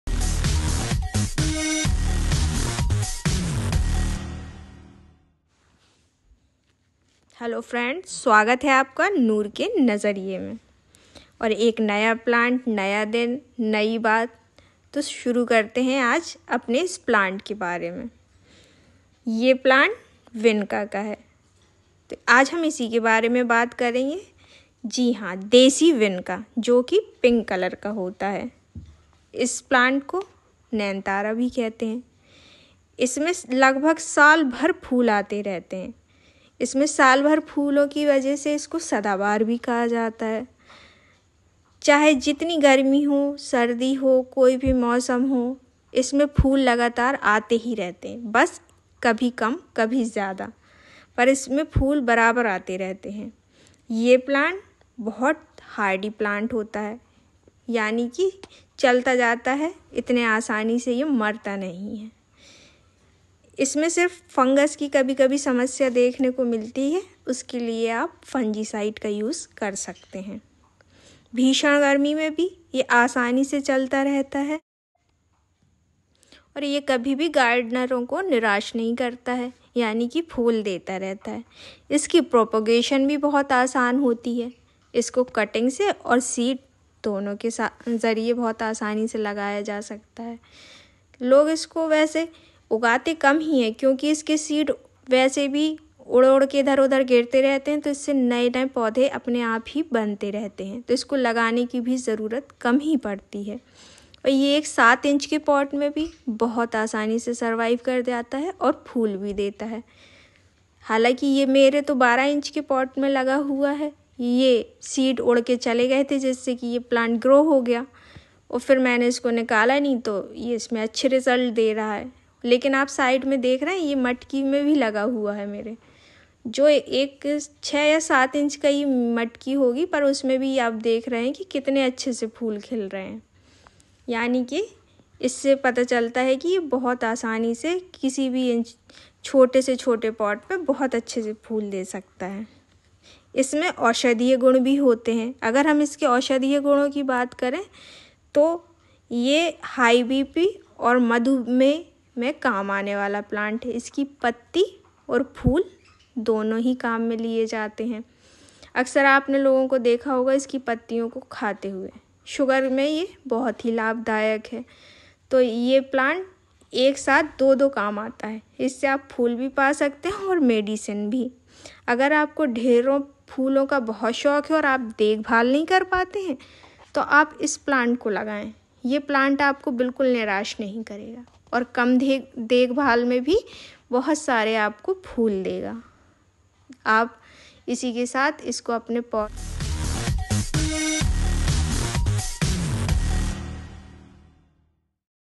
Desi vinka ! sadabahar plant k fayade! 🍀🍀
Ghar m vinka lagane ka trika aur uske fayde! jisse aapke garden m sundarta to badegi hi aur medicinal plant bhi add ho jo kaapke garden ko phoolon se bhar dega !

Noor ka 🧐 Nazariya

🙏 keep watching 😊